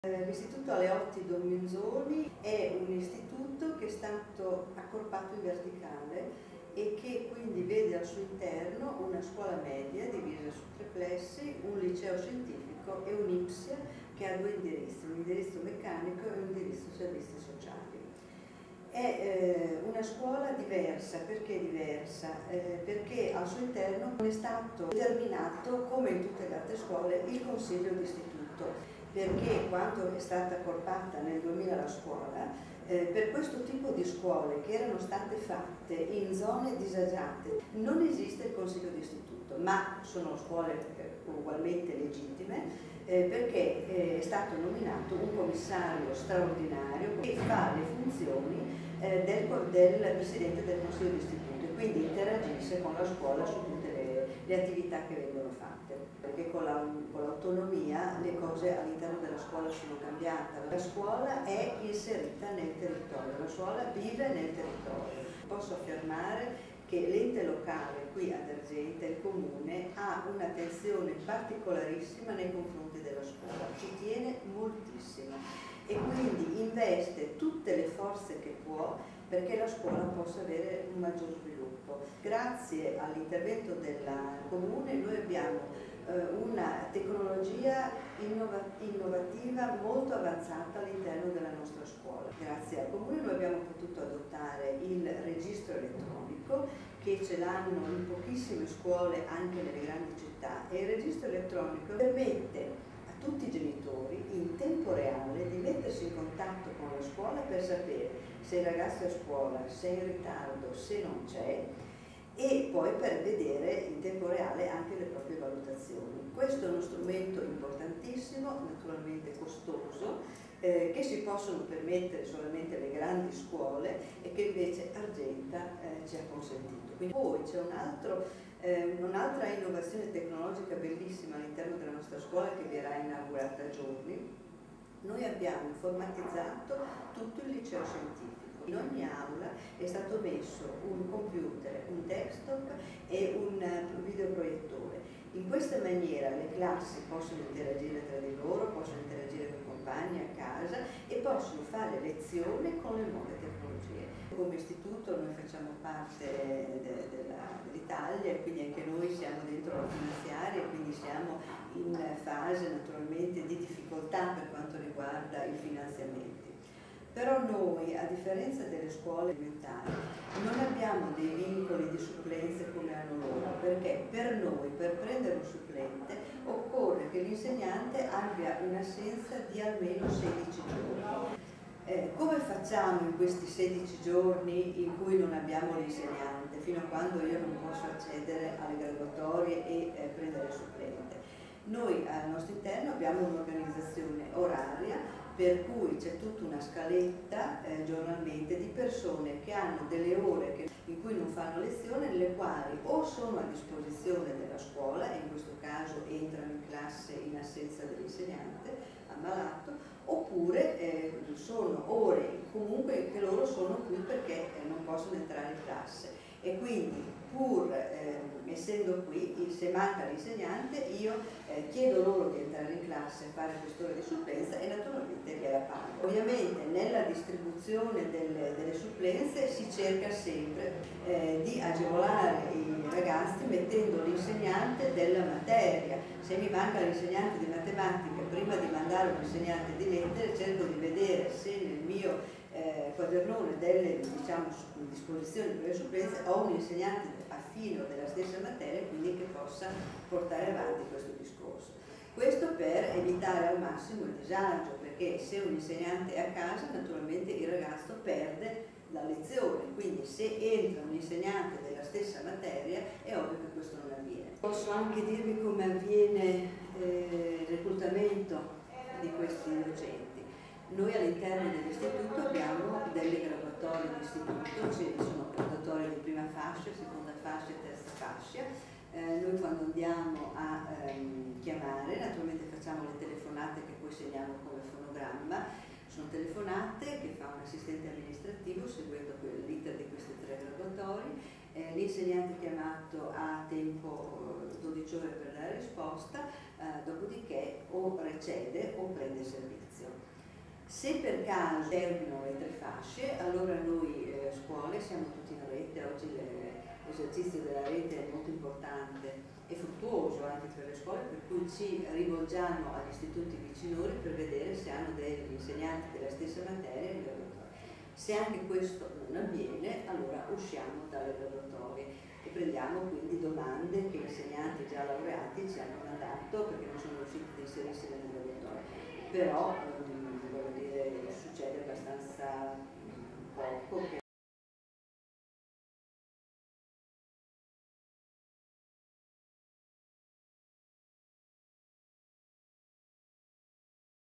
Intervista alla Preside Monti
Intervista alla dirigente scolastica, prof. Roberta Monti sulle nuove tecnologie introdotte nel Liceo Aleotti - Don Minzoni e all'IPSIA.